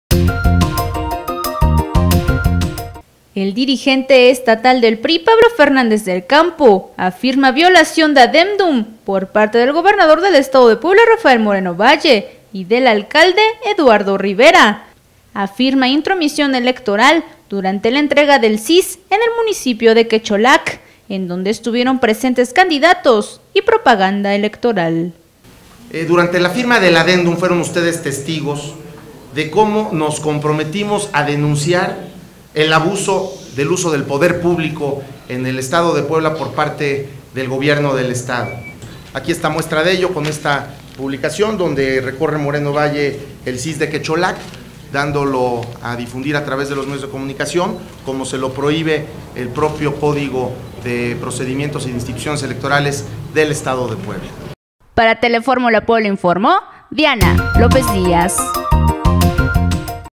PABLO FERNANDEZ DEL CAMPO VIOLA ADENDUM RAFAEL MORENO VALLE
El Dirigente Estatal del PRI Pablo Fernández del Campo afirma violación de Adéndum por parte del Gobernador del Estado de Puebla Rafael Moreno Valle y del Alcalde Eduardo Rivera, afirma intromisión electoral durante la entrega del CIS en el Municipio de Quecholac, en donde estuvieron presentes candidatos y propaganda electoral.